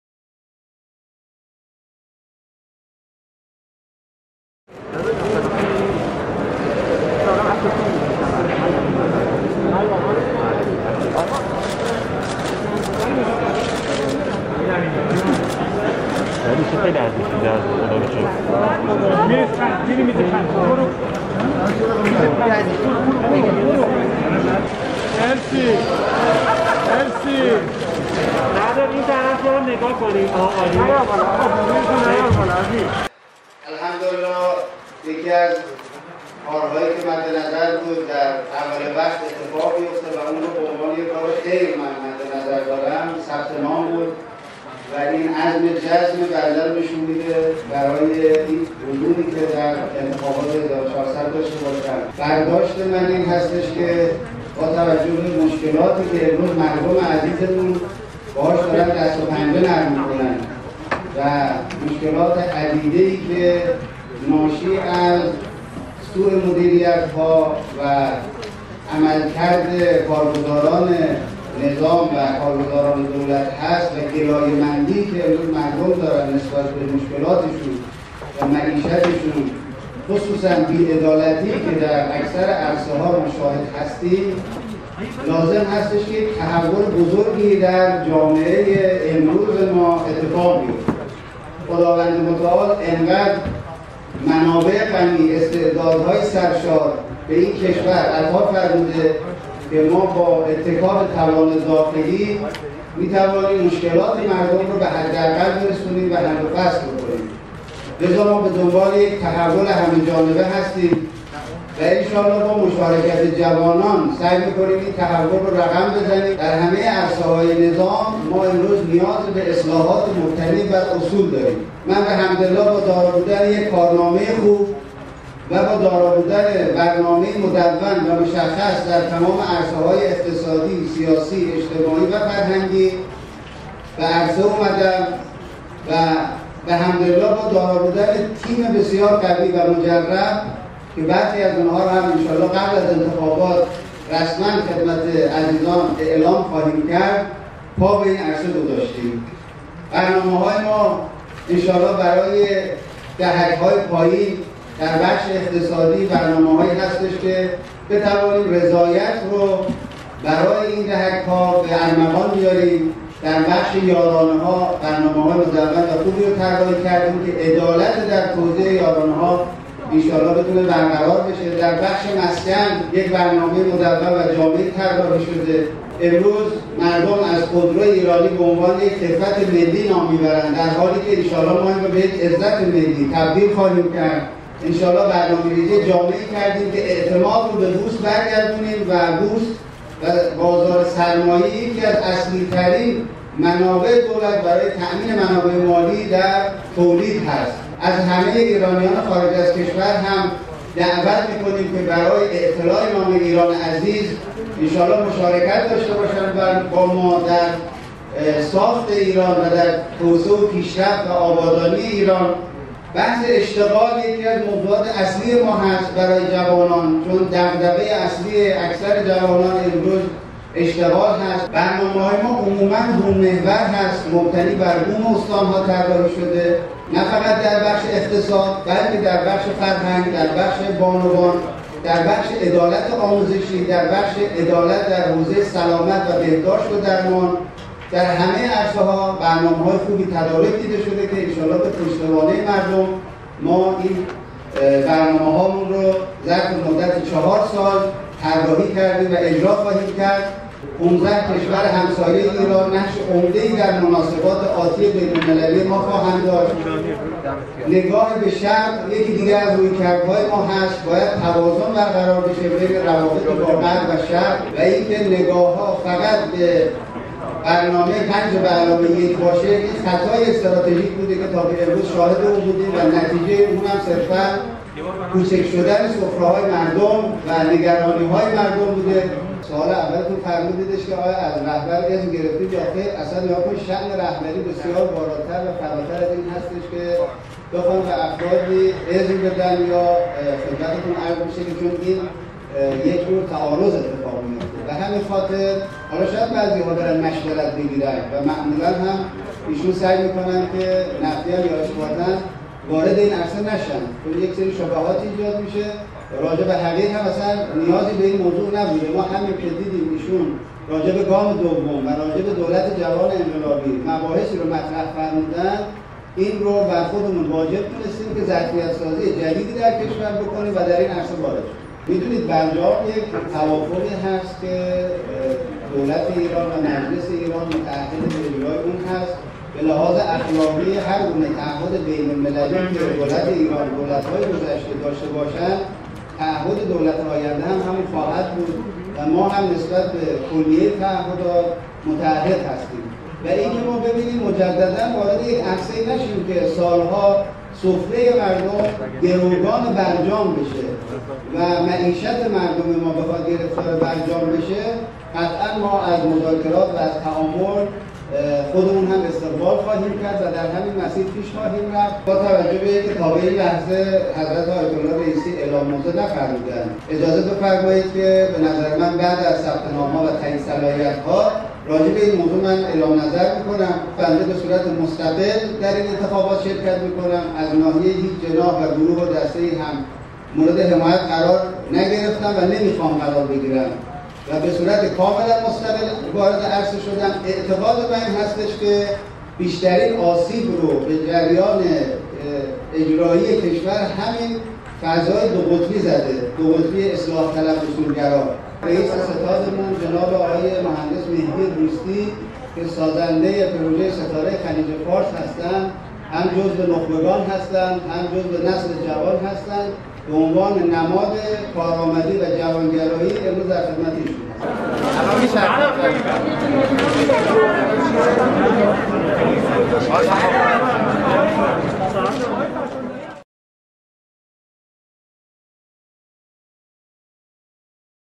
موضع «سعید محمد» درمورد احتمال کناره‌گیری به نفع رئیسی
سعید محمد با بیان اینکه «به صورت مستقل در انتخابات شرکت می کنم»، اظهار کرد: از ناحیه هیچ جناحی هم مورد حمایت قرار نگرفتم و نمی خواهم هم که مورد حمایت آنها قرار بگیرم. او که امروز برای انتخابات ریاست جمهوری ثبت‌نام کرده، در نشست خود در وزارت کشور در پاسخ به پرسشی درباره احتمال کناره‌گیری‌اش به نفع رئیسی گفت: با توجه به اینکه آیت‌الله رئیسی اعلام موضع نفرمودند و همچنین با توجه به اینکه کارزار انتخابات هنوز شفاف نیست، پس از پایان ثبت‌نام و تأیید صلاحیت‌ها در این زمینه اعلام‌نظر می‌کنم.